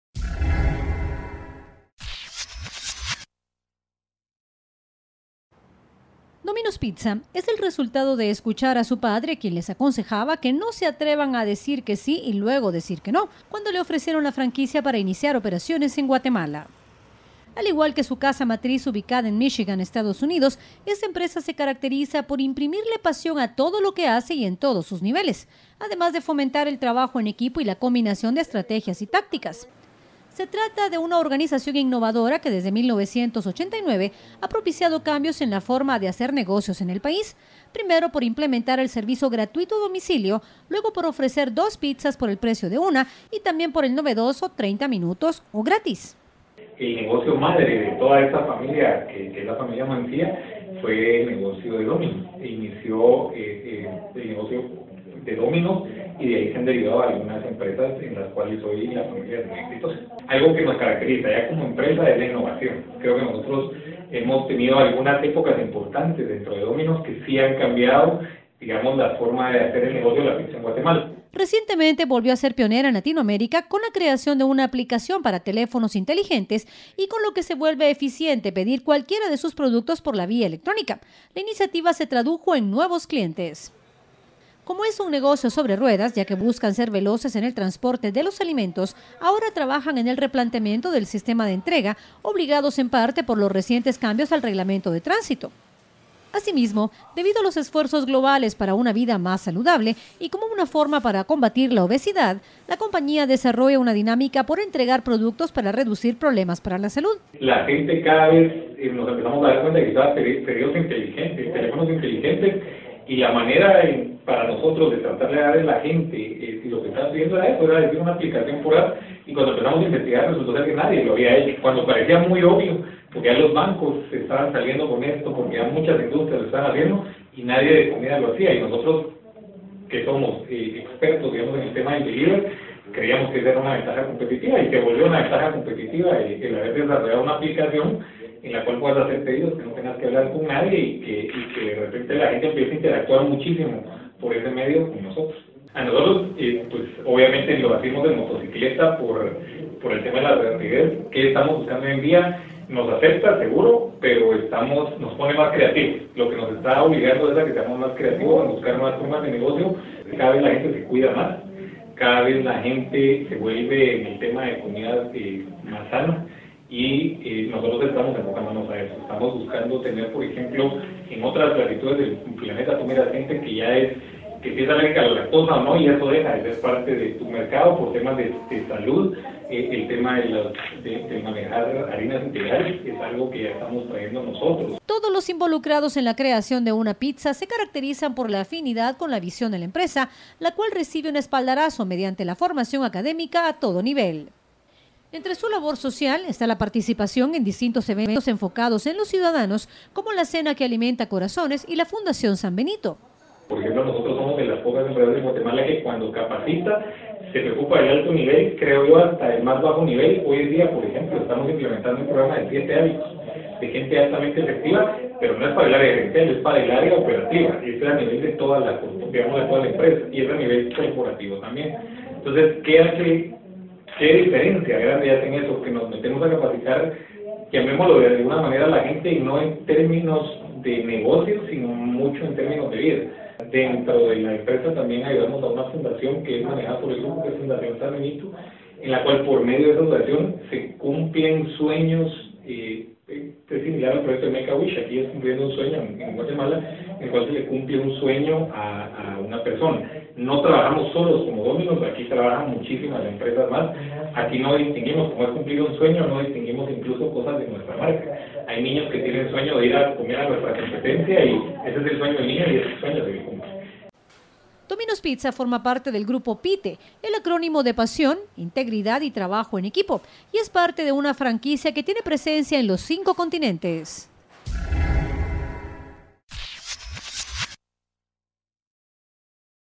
CEO a CEO - Domino's Pizza
Conversamos con el CEO  de esta empresa famosa por sus "30 minutos o gratis", su oferta del "2x1" y, por supuesto, la calidad de su producto. ¿Cómo lo logran? He aquí su fórmula.

Por: Juan José López Torres